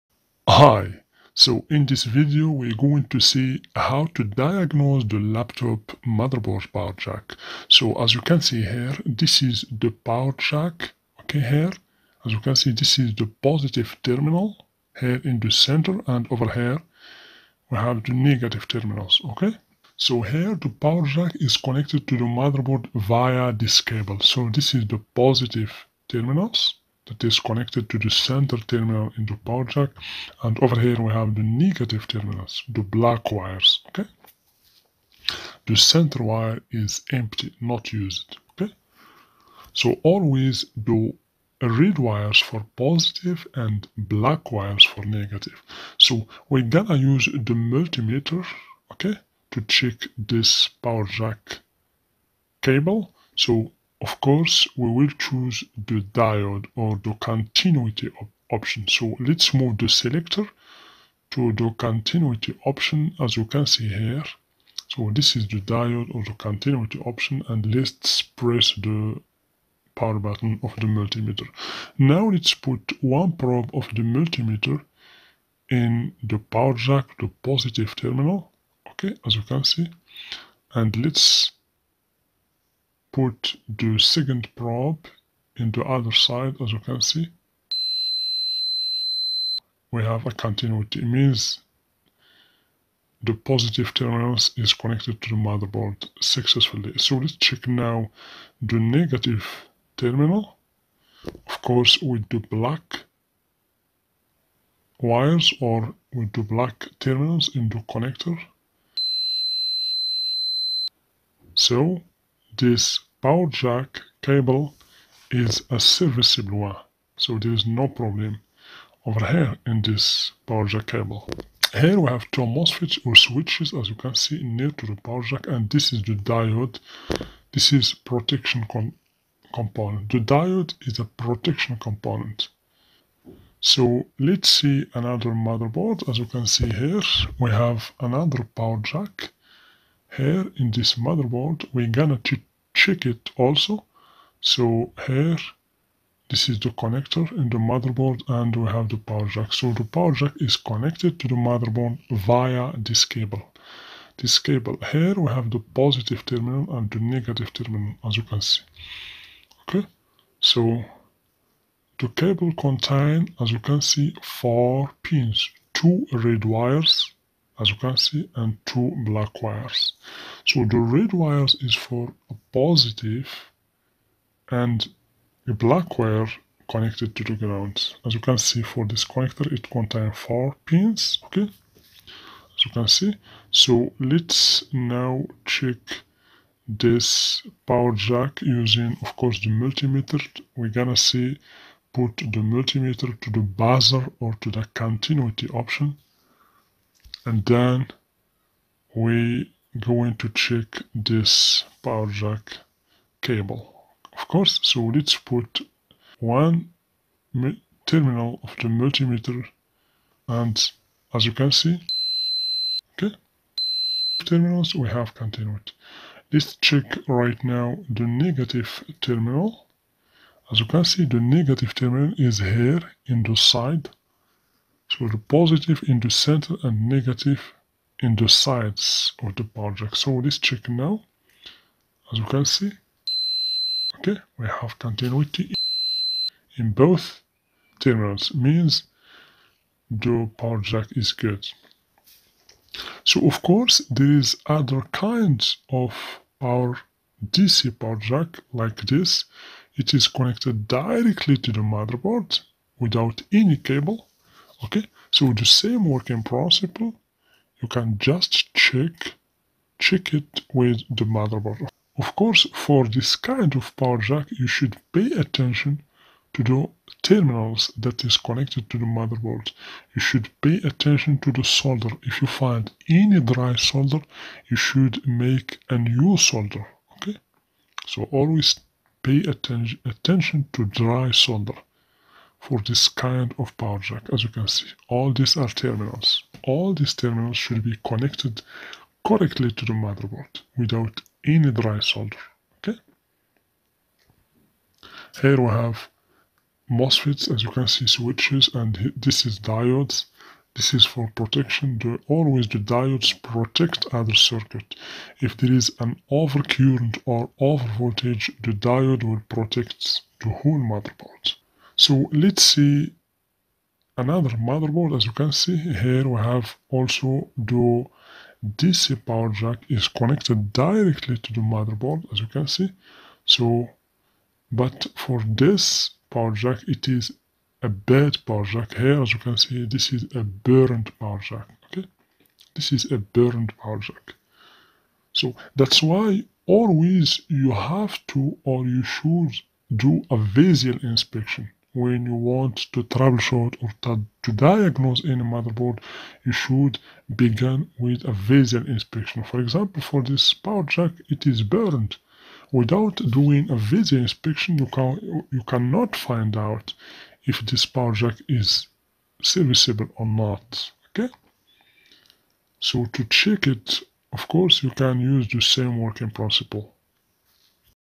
Learn how to Diagnose Laptop DC Power Jack Port
In this video, you going to learn how to troubleshoot the laptop's dc power jack & repair it.